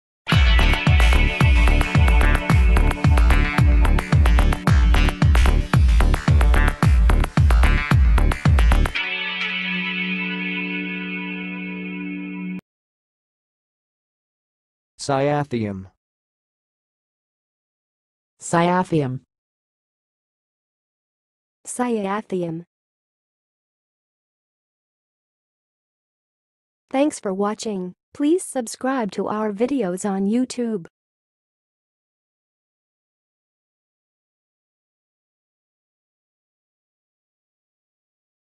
CYATHIUM - HOW TO PRONOUNCE IT?
CYATHIUM - Mastering the pronunciation of Cyathium in 2023

How to pronounce 'cyathium'?
What is the correct pronunciation of 'cyathium'?
Could you provide alternative pronunciations for 'cyathium'?
CYATHIUM Meaning And Definition:
A cyathium is a specialized inflorescence found in certain plants, particularly in the family Euphorbiaceae.
It consists of a cup-shaped structure called a cyathium, which encloses several small flowers.
The cyathium has a central female flower surrounded by male flowers that are reduced in size.
The cyathium is often surrounded by bracts that may be brightly colored or modified to attract pollinators.
This unique structure is characteristic of plants in the Euphorbiaceae family and plays a crucial role in their reproduction.
CYATHIUM Pronunciation:
To pronounce 'cyathium', break it down into syllables: cy-a-thee-um.
The emphasis is on the second syllable, 'a'.
You can also try pronouncing it as 'sigh-AY-thee-um' or 'sigh-AH-thee-um' if that feels more comfortable.
Use CYATHIUM In Your Speech:
1. The cyathium is a fascinating flower structure found in plants of the Euphorbiaceae family
2. The brightly colored bracts surrounding the cyathium attract pollinators
3. The unique reproductive strategy of plants with cyathia has evolved to ensure successful reproduction
Please answer in comments for the most popular CYATHIUM questions:
1. What are some examples of plants that have cyathium as their inflorescence structure?
2. How do the bracts surrounding the cyathium help in pollination?
3. What is the role of the central female flower in the cyathium?